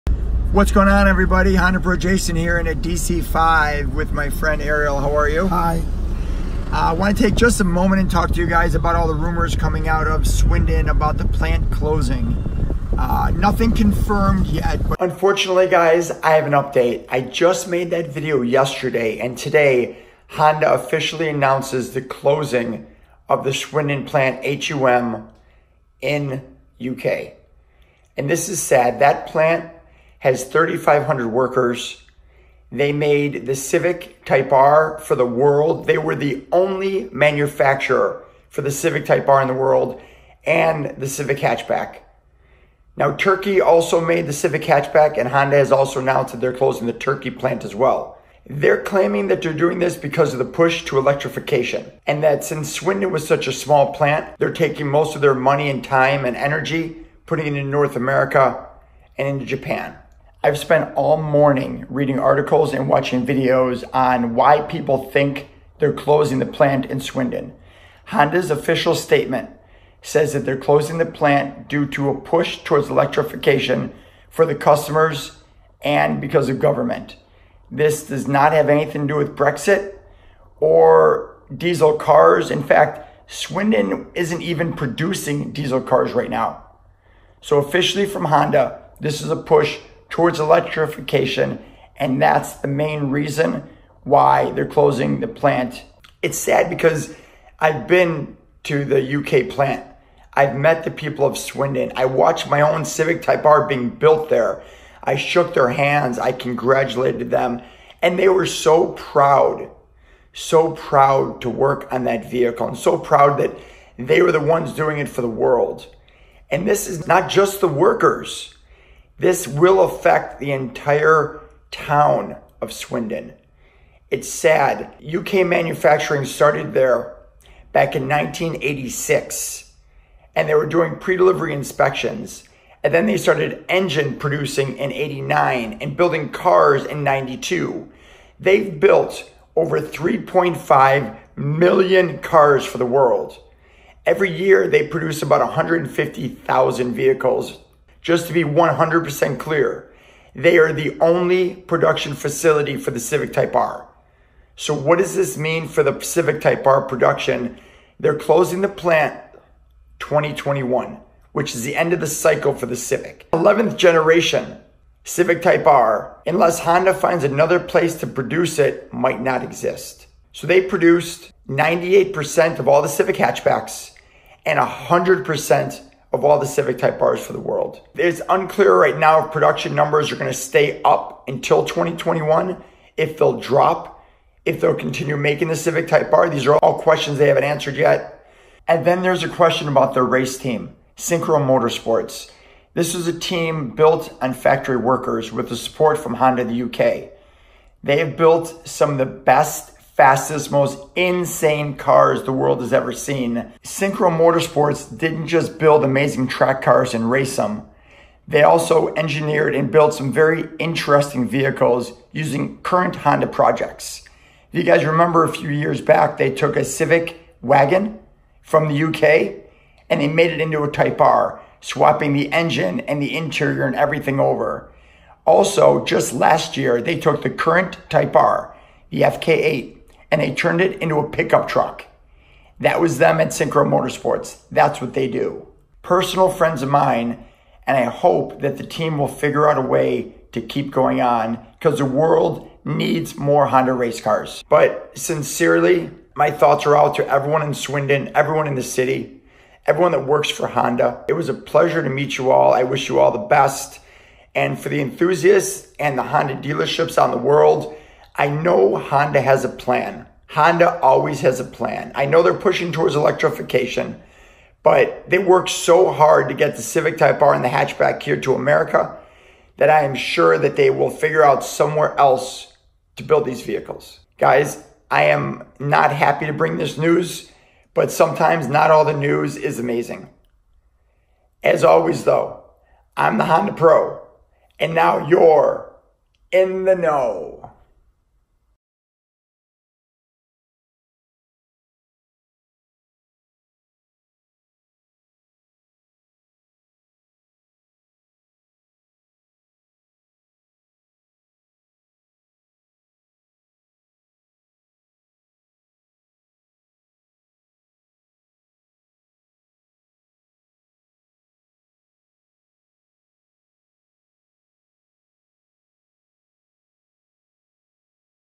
Honda Announces the Closing of The Only Factory for the Civic Type R
Honda announces the closing of the Swindon Plant (HUM) Honda UK Manufacturing. This plant where 100% of the Worlds Civic Type R's are built will close in 2021 at the end of production of the current generation.  This is where the Honda Civic Hatchback is also produced, with 3500 associates and many more parts makers and local people being effected.  In this video I talk about why and about my visits to HUM and Synchro motorsport. Honda claims the push for electric cars is most of the reason for the shut down.